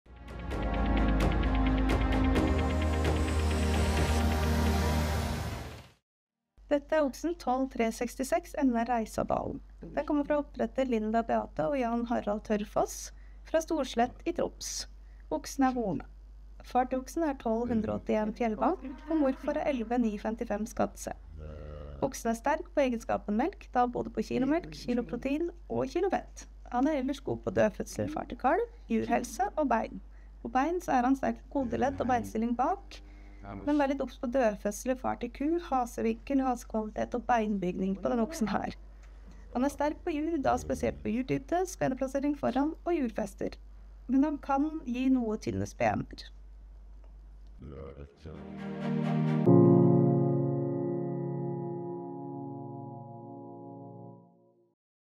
12366 NR Reisadalen
Video av NRF-oksen 12366 NR Reisadalen
Oppdretter: Linda Beate og Jan-Harald Tørfoss

Følg oss gjerne på sosiale medier!